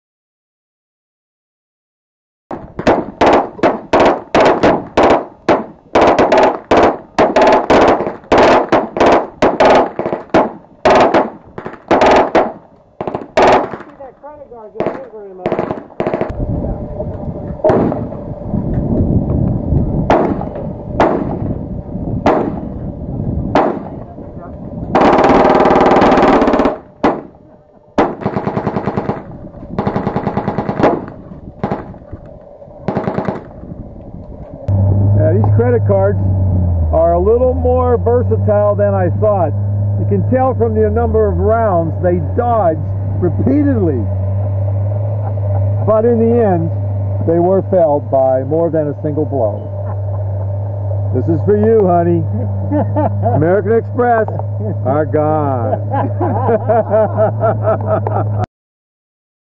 Machine Gun Plastectomy
Destruction of credit cards using machine guns.  Debt free living.